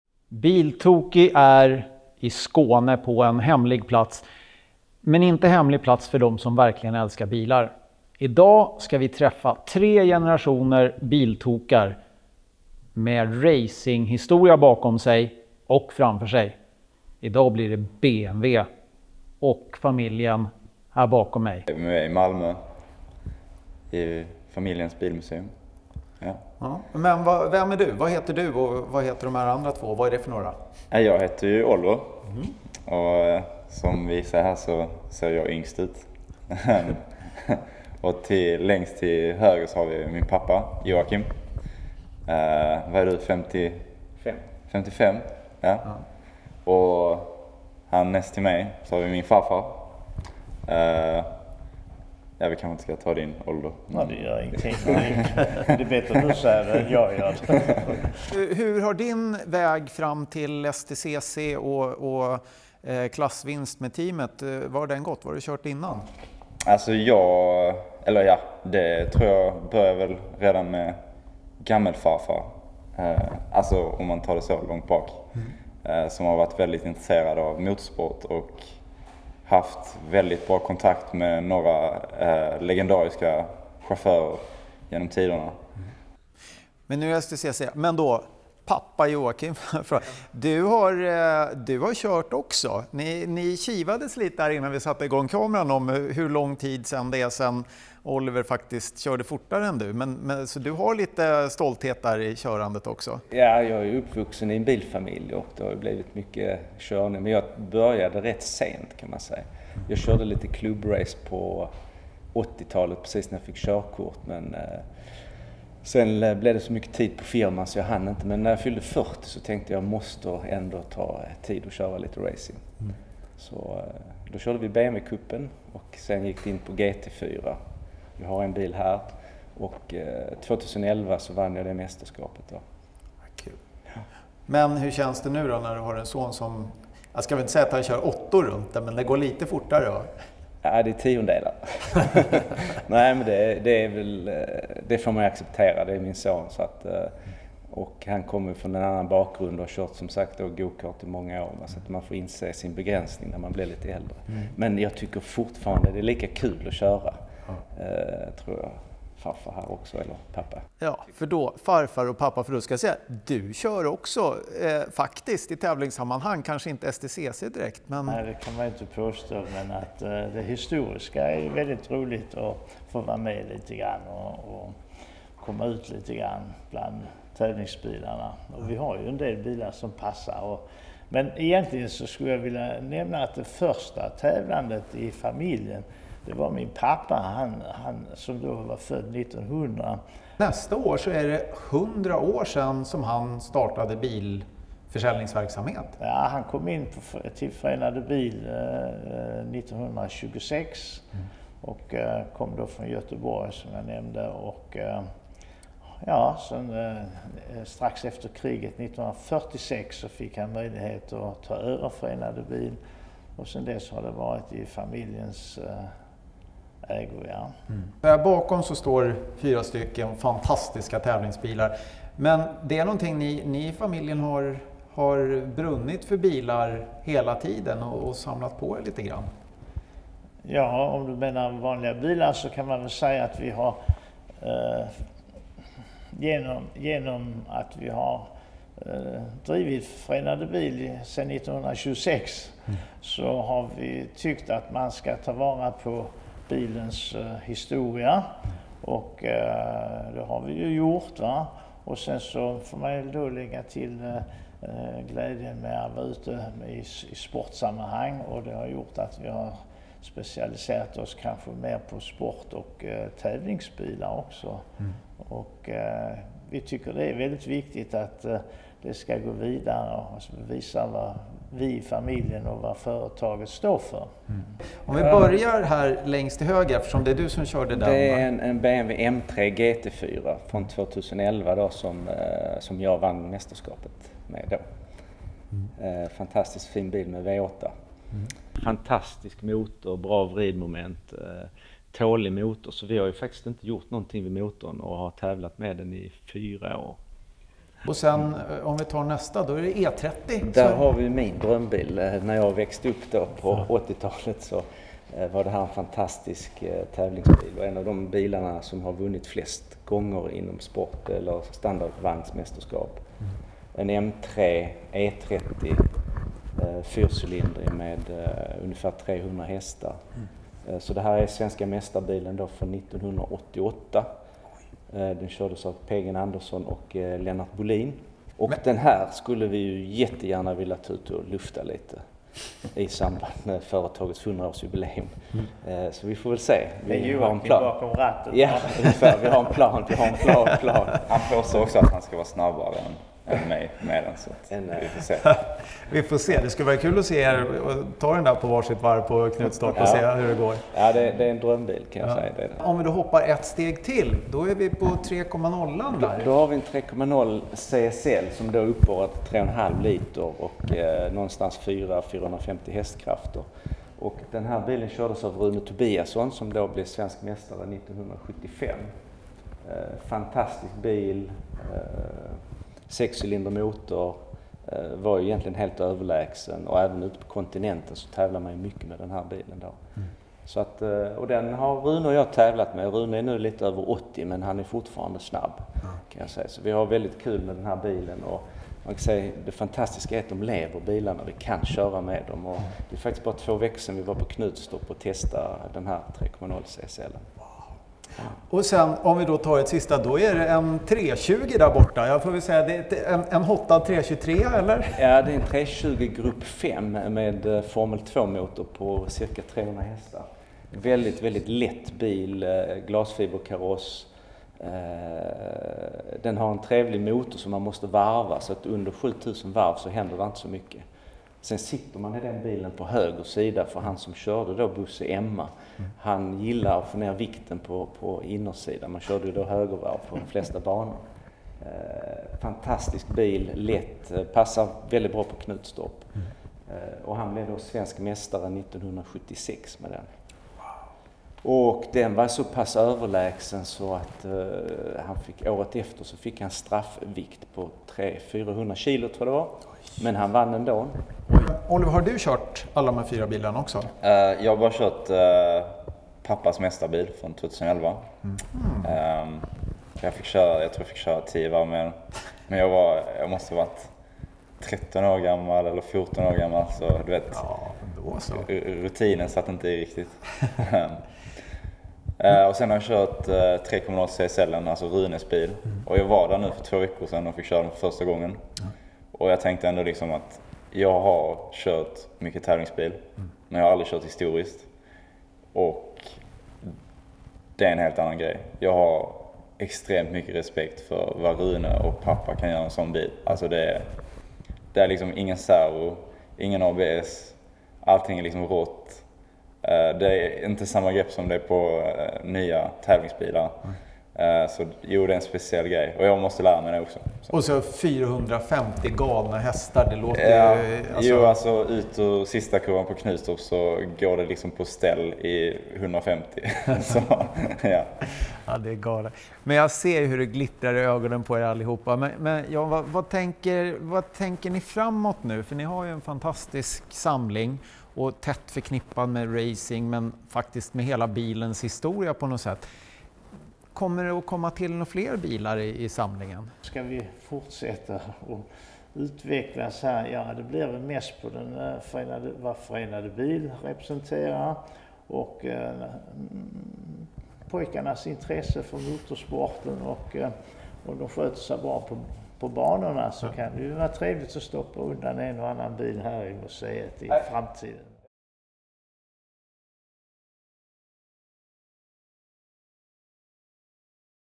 Familjen Söderströms BMW-samling
Vissa familjer är mer Biltokiga än andra. Familjen Söderström i Malmö har varit bilhandlare i 100 år och lika länge har de samlat unika bilar. Det har resulterat i en fantastisk samling och Biltokig fick ett snack med tre generationer Söderströmska Biltokar och en titt på fyra unika BMW-racers.